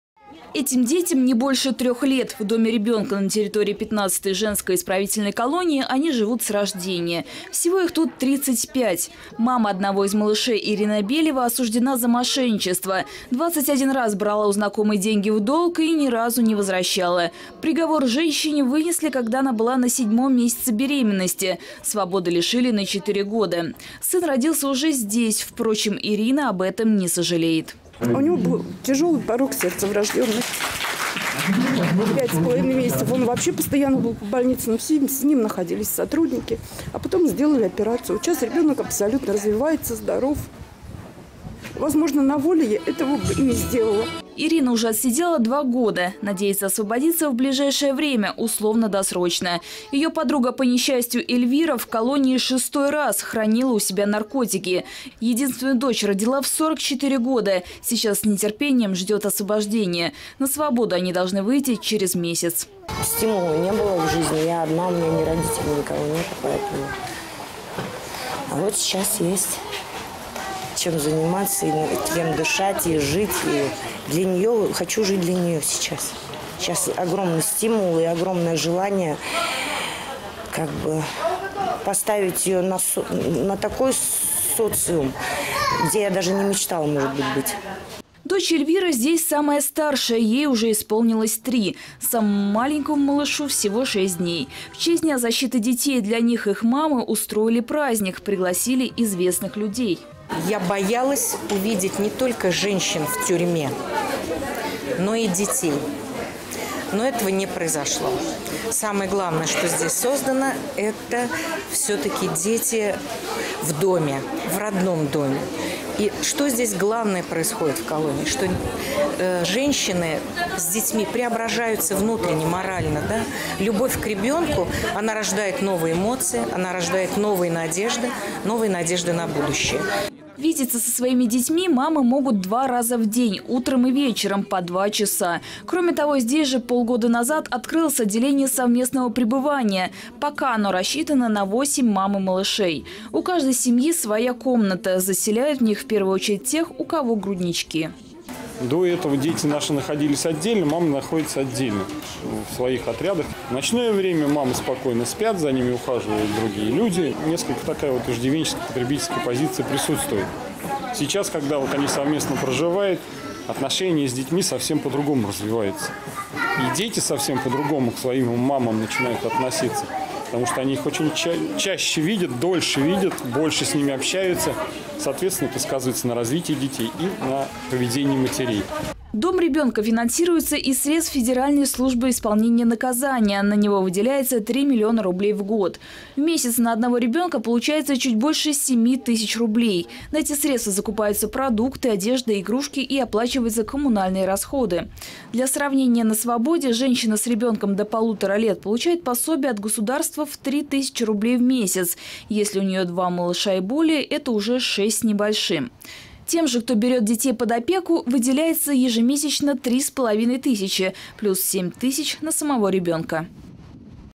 Дети в колонии
В первый день лета отмечается еще один, относительно новый, праздник, появившийся в 2013 году - это Всемирный день родителей. Которых, как известно, не выбирают. И тут, справедливости ради, хочется вспомнить народную мудрость :"От сумы и от тюрьмы не зерекайся". По данным ФСИН, ежегодно в России за колючей проволокой появляются на свет около 260–300 младенцев. В Самарской области дом ребенка есть только в одной колонии. Сейчас там живут 35 детей. Матери большинства из них осуждены за хранение наркотиков, впрочем, есть и отбывающие наказание за убийство. Об условиях содержания малышей в таких заведения и о том, какие плюсы видят некоторые женщины в беременности за решеткой, сюжет Светланы Бессольцевой.